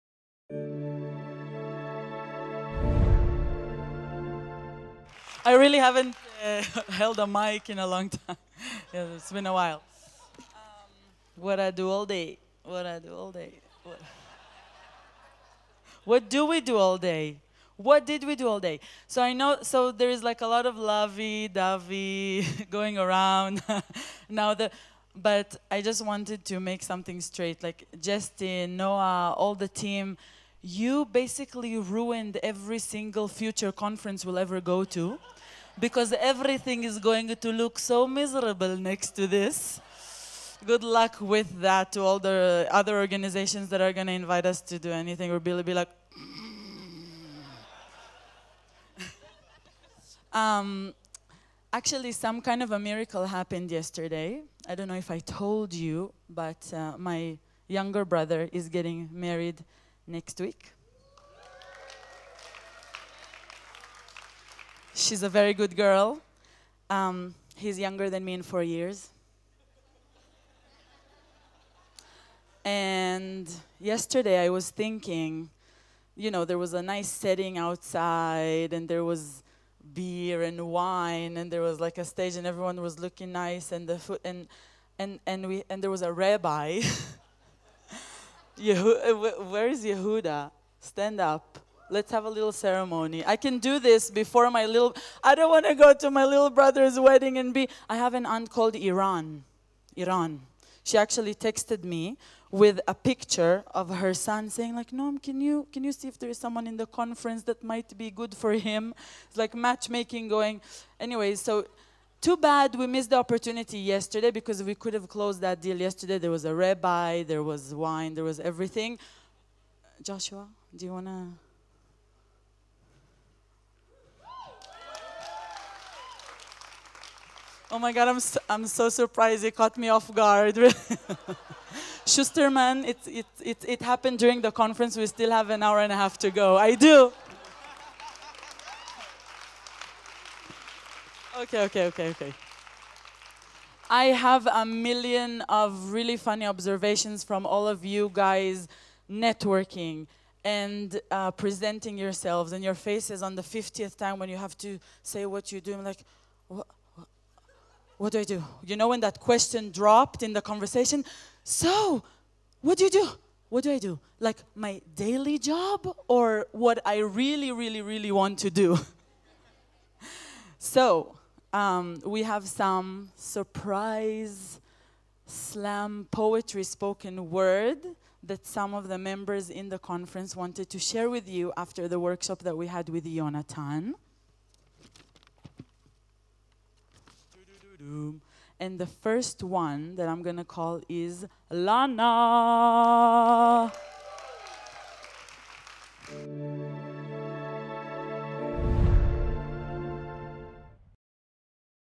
2017 ROI Summit Closing Ceremony - Noam Shuster Introduces Spoken Word
Noam Shuster opens the spoken word segment of our 2017 ROI Summit closing ceremony with a bit of storytelling. Watch!

The Summit, ROI’s flagship program, is the gateway into ROI Community.